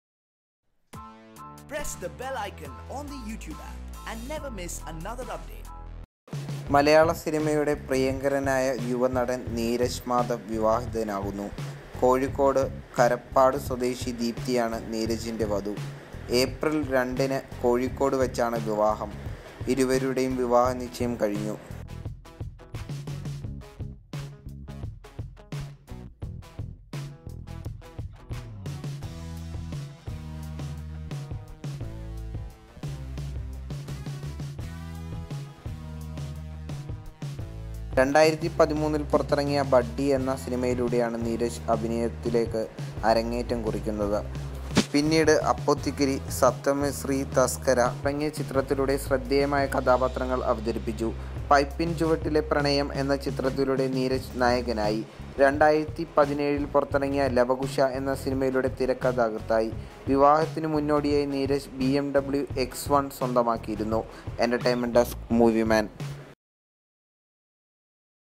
Neeraj Madhav Wedding Engagement Video , Photo , Teaser
Neeraj Madhav is an Indian film actor and dancer who works in Malayalam films. Neeraj made his debut in Malayalam through the film Buddy directed by Raaj Prabavathy Menon